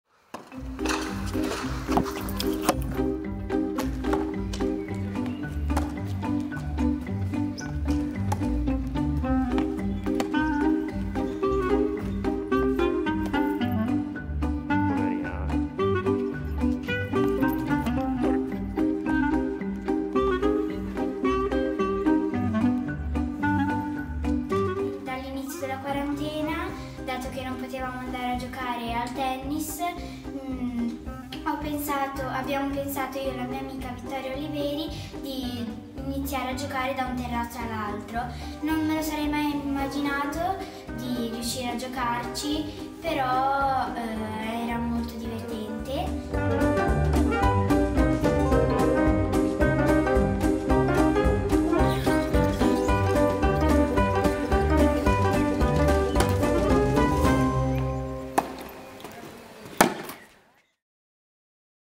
Quadras nos telhados | AFP
Com as barreiras como rede e os telhados dos edifícios como quadra, duas tenistas driblam o isolamento social para continuar treinando longe do perigo da contaminação pelo novo coronavírus no norte da Itália. VERSÃO LEGENDADA